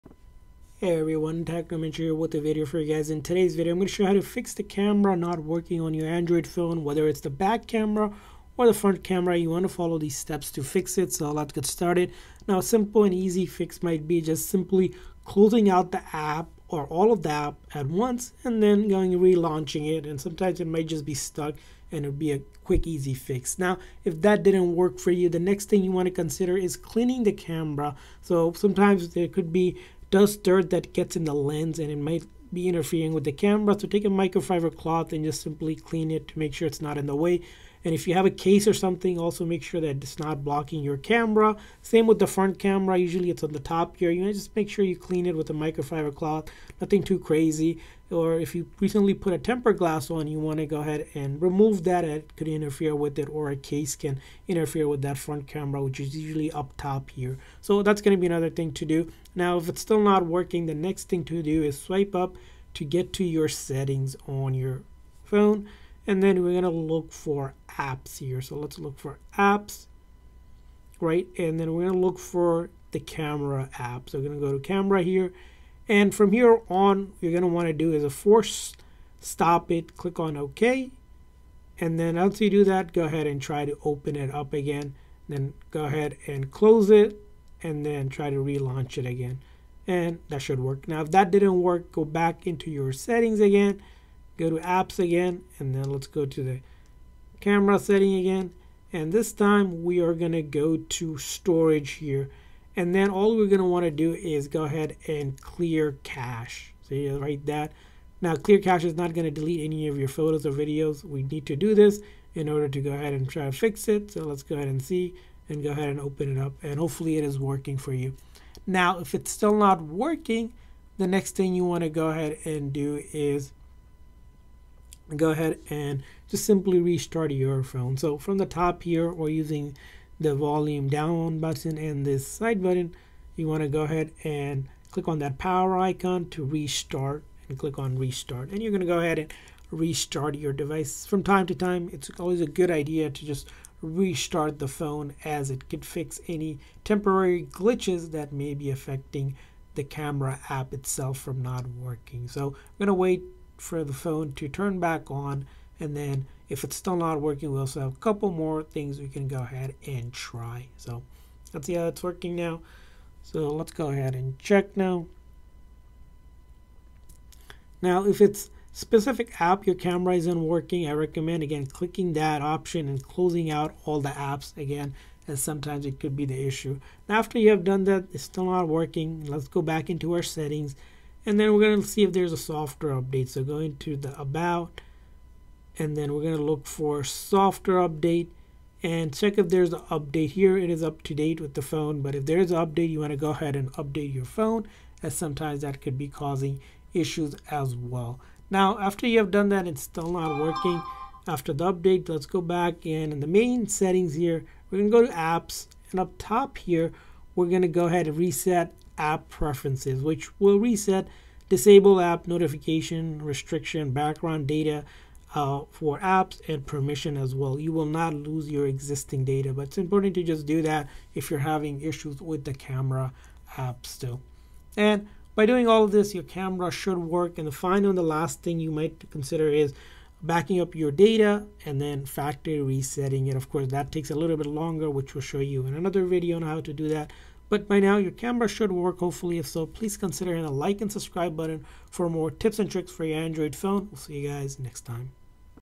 How To Fix Camera Not Working On Android Phone
If the camera is not working on your Android or Samsung phone, you can follow these steps to fix it!